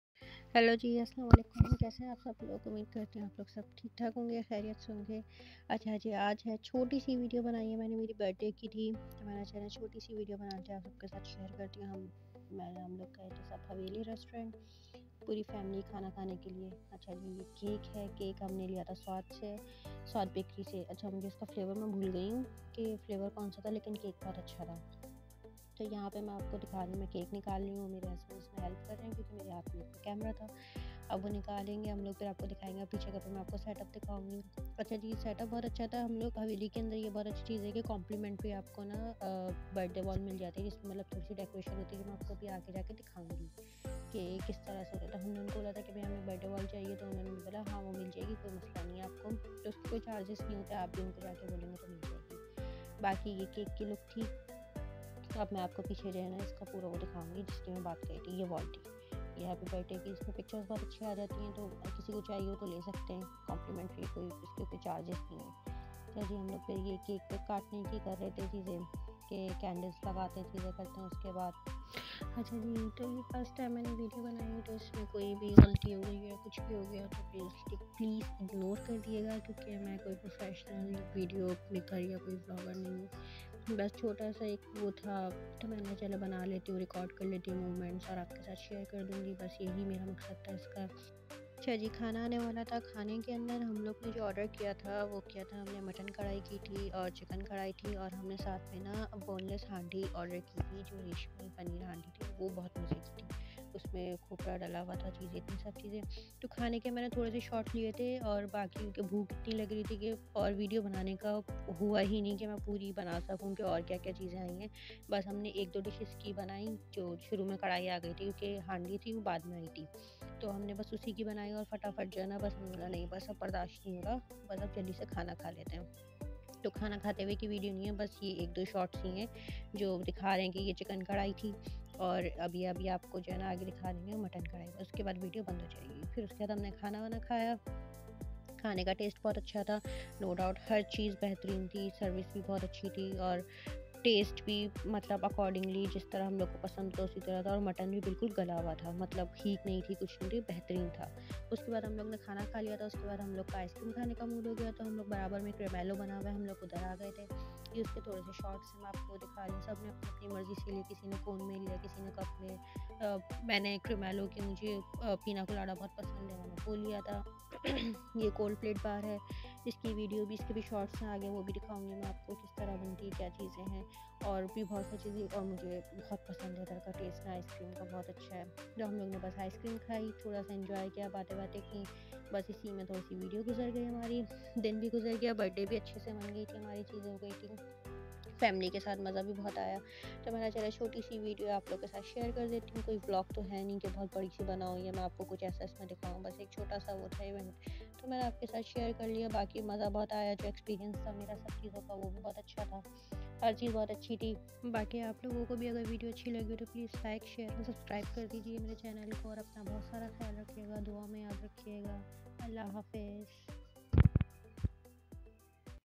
My Birthday celebration at Haveli Restaurant | Birthday party 🎉
haveli restaurant tastes 10/10
service 8/10
ambiance 10/10
The food was good especially the mutton that was cooked very well.
then we visited creamello and ordered ice creams , shakes , faloda and enjoyed the ice cream in winter.
The birthday celebration was awesome and memorable 🎈🎉.
🎂🍧🍨🍦🍰 happy birthday Haveli restaurant karachi haveli restaurant karachi 5 star Creamello karachi Suaad bakery for you sweets videos